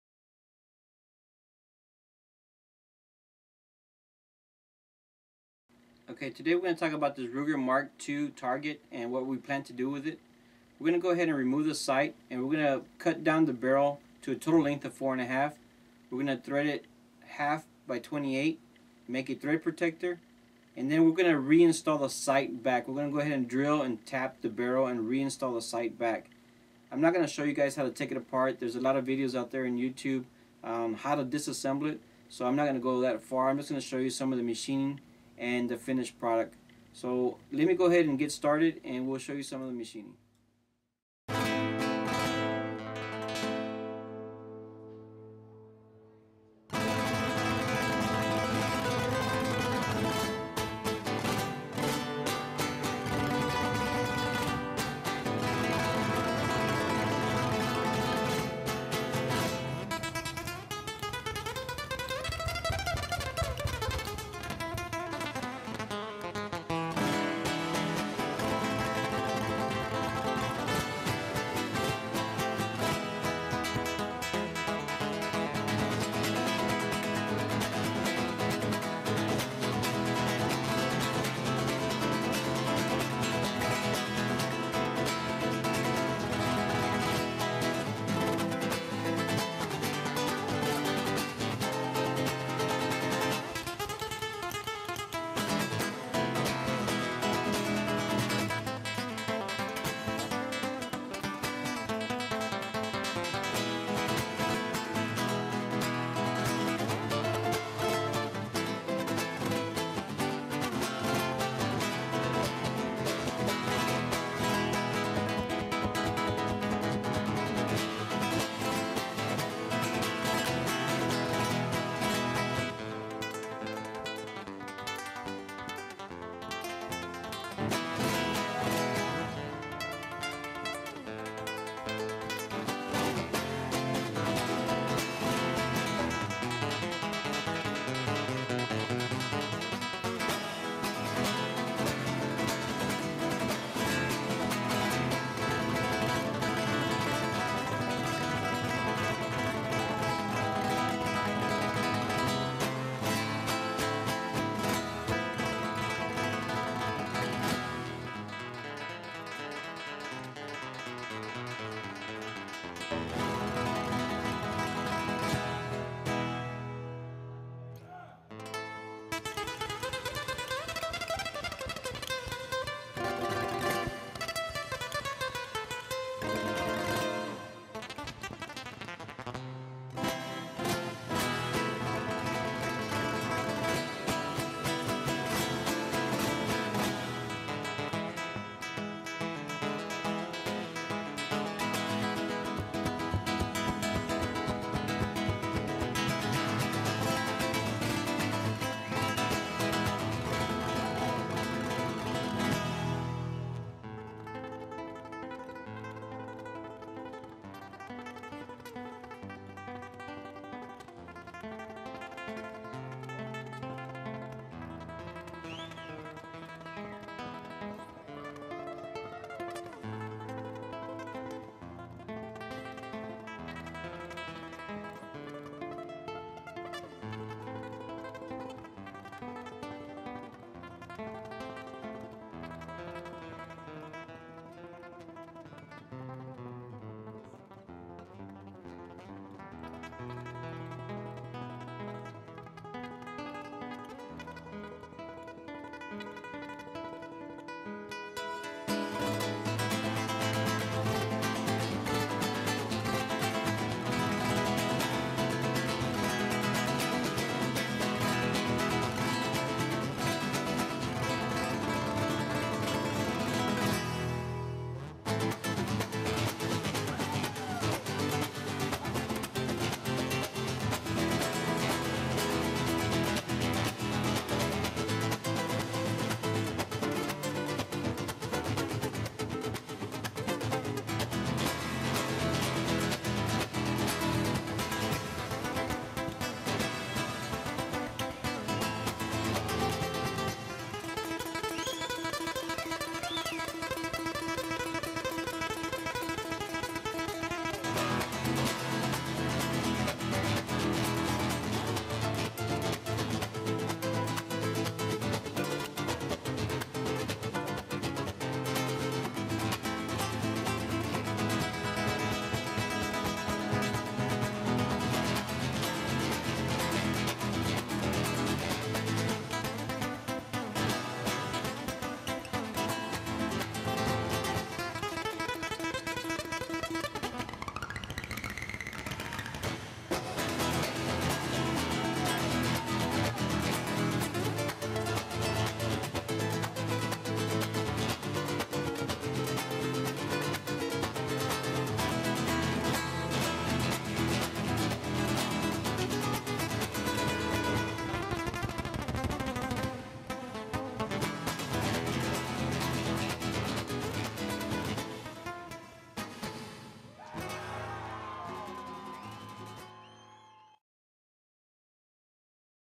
Threading Barrel Ruger MKII Target Pistol
This video demonstrates the machining and threading of a Ruger MKII Target Pistol Barrel.